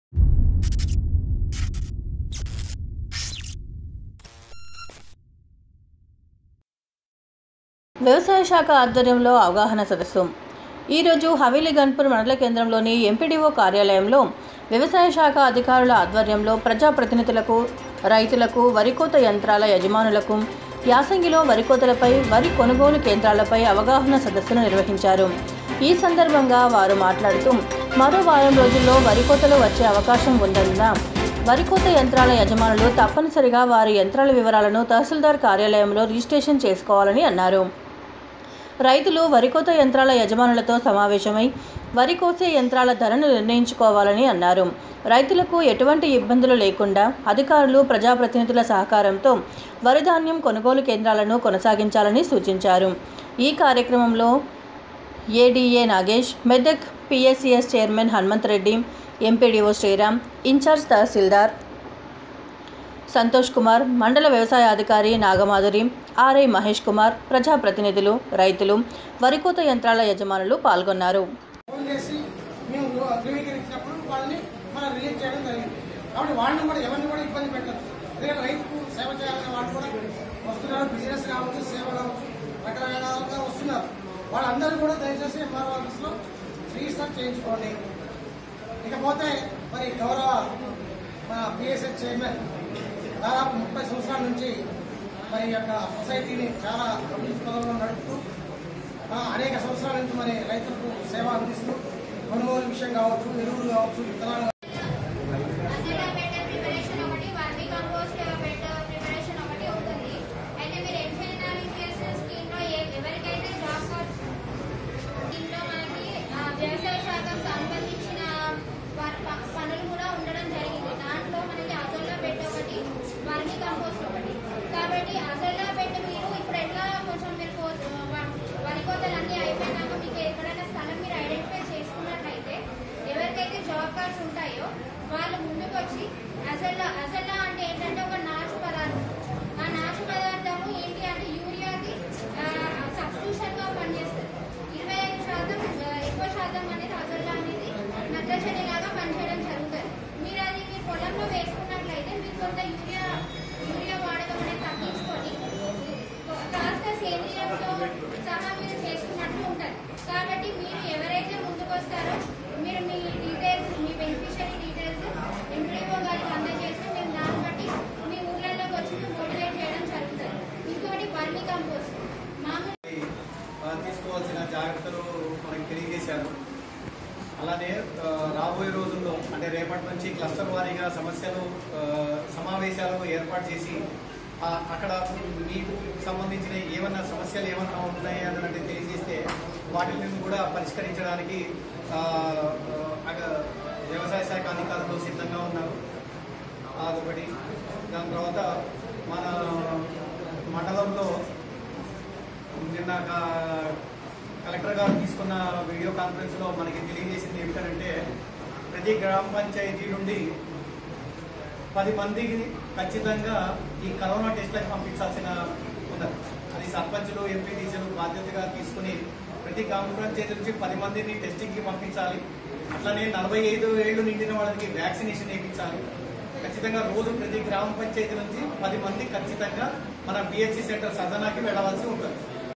THIRUMALA NEWS 07 04 2021
ఈరోజు హావేలి ఘణాపూర్ మండల కేంద్రంలోని ఎంపీడీఓ కార్యాలయంలో వ్యవసాయ శాఖ అధికారుల ఆధ్వర్యంలో ప్రజాప్రతినిధులకు, రైతులకు, వరికోత యంత్రాల యజమానులకు,యాసంగిలో వరికోతలపై,వరి కొనుగోలు కేంద్రాలపై అవగాహన సదస్సు నిర్వహించారు